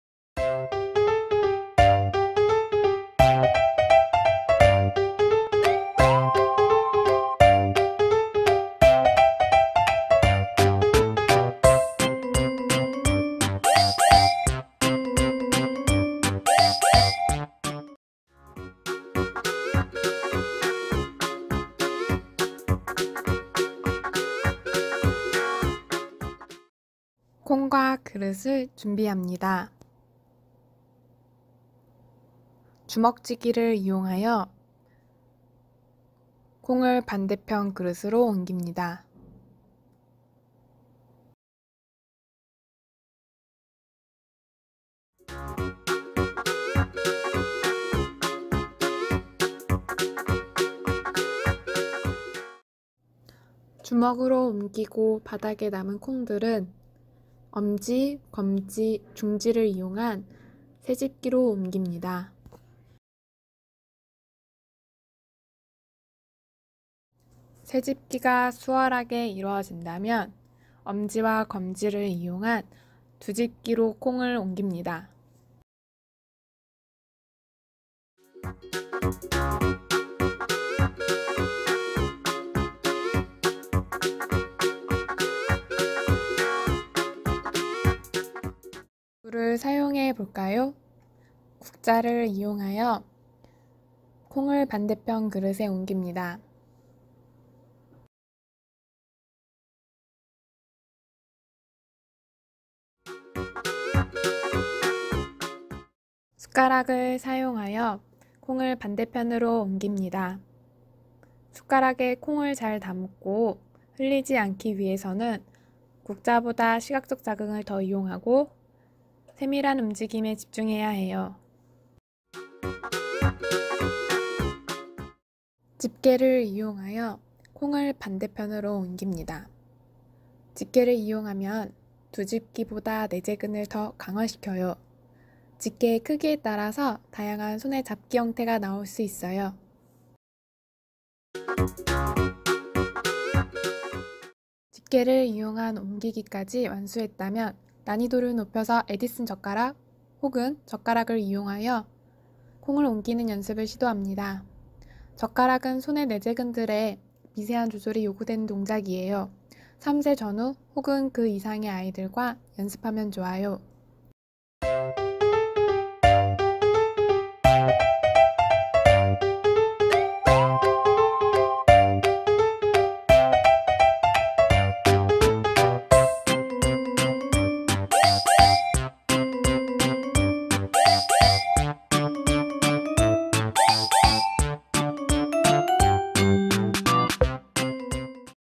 운동기능향상실 - 콩을 이용한 다양한 활동
가정 내에서 손의 다양한 잡기 및 집기 형태를 이용하여
내재근 및 외재근 발달을 유도한 활동입니다.

콩을 주제로 사용하였으며
상황에 따라서 시각, 촉각, 청각을 자극하기도 합니다.

가정 내에서 흔히 볼 수 있는 도구를 사용하므로
쉽게 따라 할 수 있는 운동 및 놀이입니다.